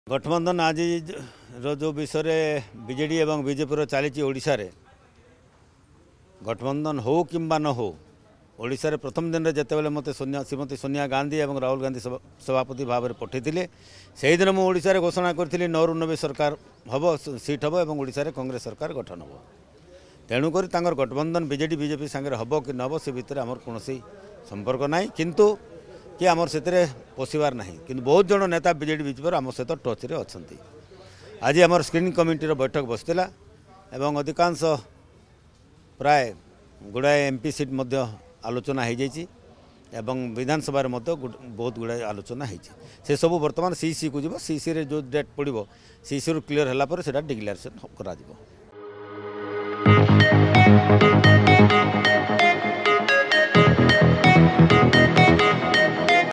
‘ଗଠବନ୍ଧନ ହେଉ କି ନ ହେଉ, ଆମର 9 ରୁ 90’ | Odisha Reporter
ବିଜେପି-ବିଜେଡି ଗଠବନ୍ଧନ ହେଉ କି ନହେଉ, ସେଥିରେ ଆମର କିଛି ସମ୍ପର୍କ ନାହିଁ। ମୁଁ ପିସିସି ସଭାପତି ହେଲା ଦିନରୁ ଘୋଷଣା କରିଛି। କଂଗ୍ରେସ ଓଡ଼ିଶାରେ 9ରୁ 90 ହେବ ବୋଲି କହିଛନ୍ତି କଂଗ୍ରେସ ପିସିସି ସଭାପତିି ଶରତ ପଟ୍ଟନାୟକ।